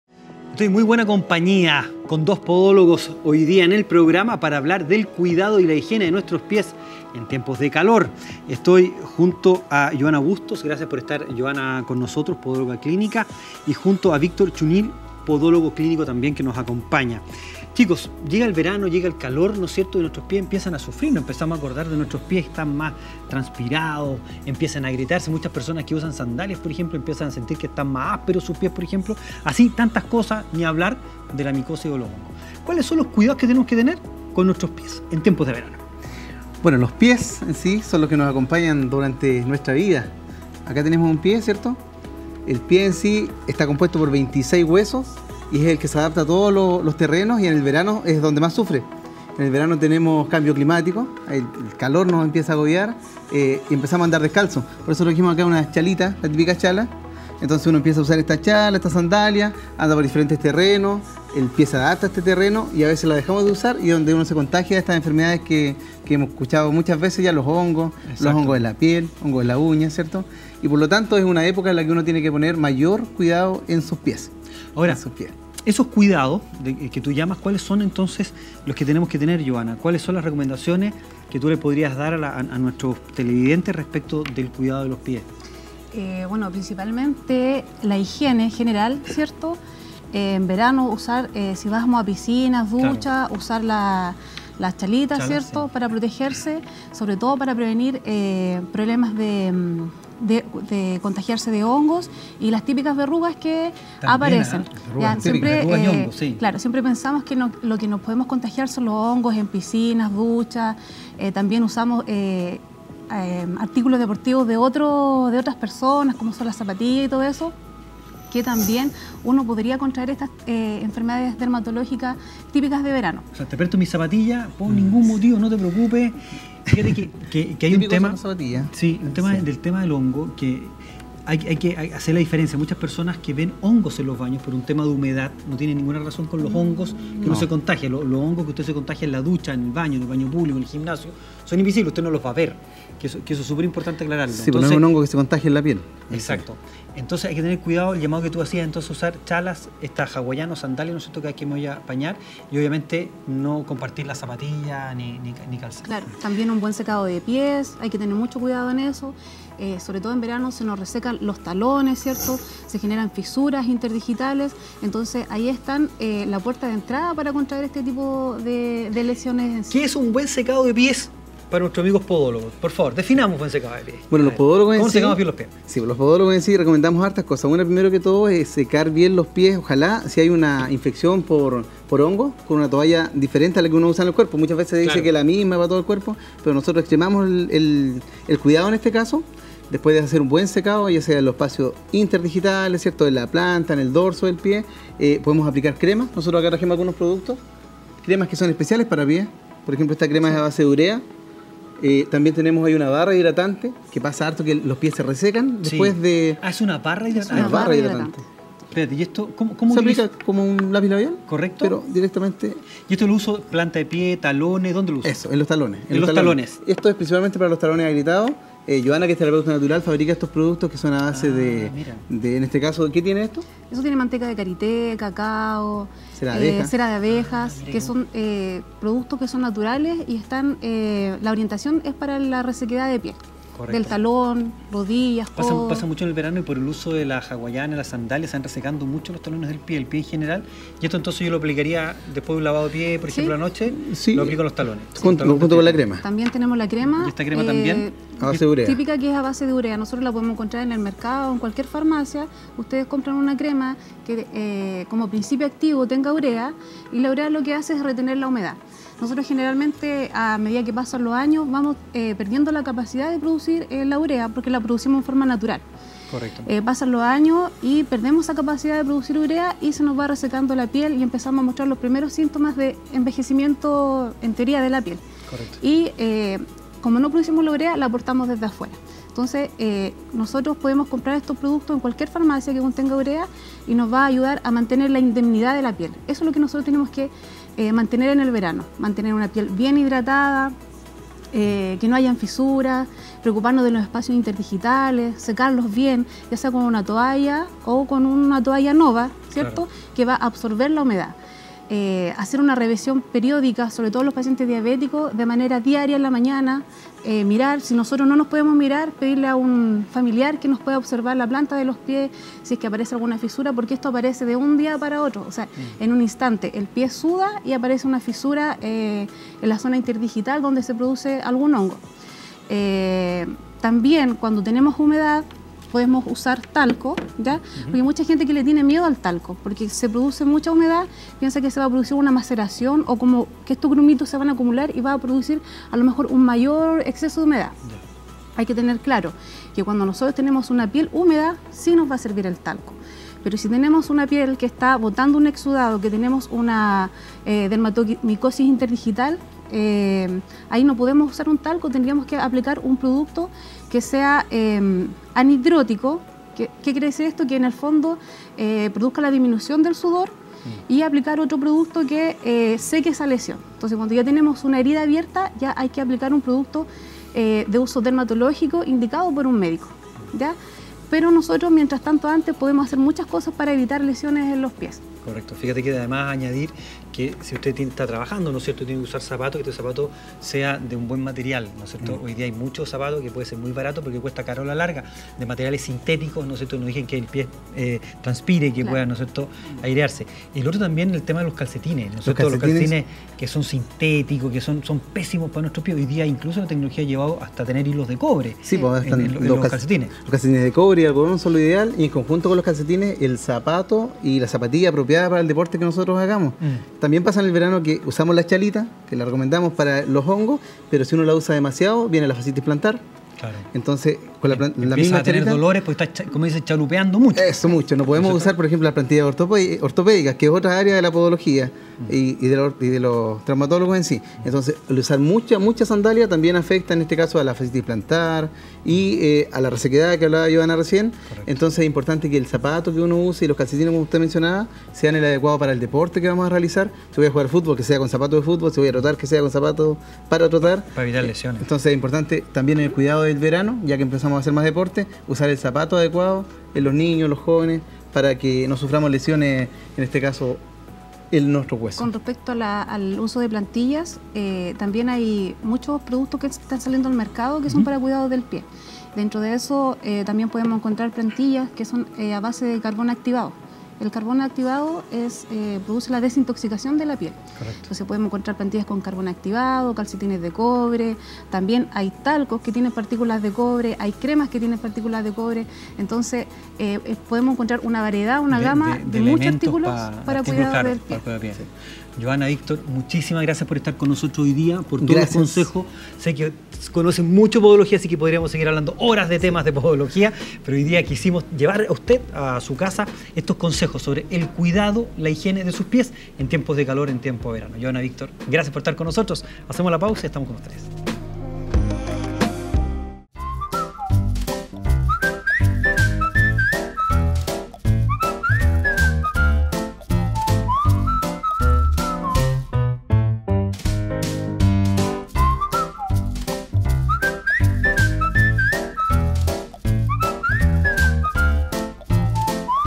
Higiene y cuidado de los pies en verano
Hoy nos acompañan Yohanna bustos y Victor Chuñil, dos podologos clínicos que de manera interactiva nos aconsejan como tener un buen cuidado e higiene de nuestros pies, considerando las altas temperaturas que hay este verano.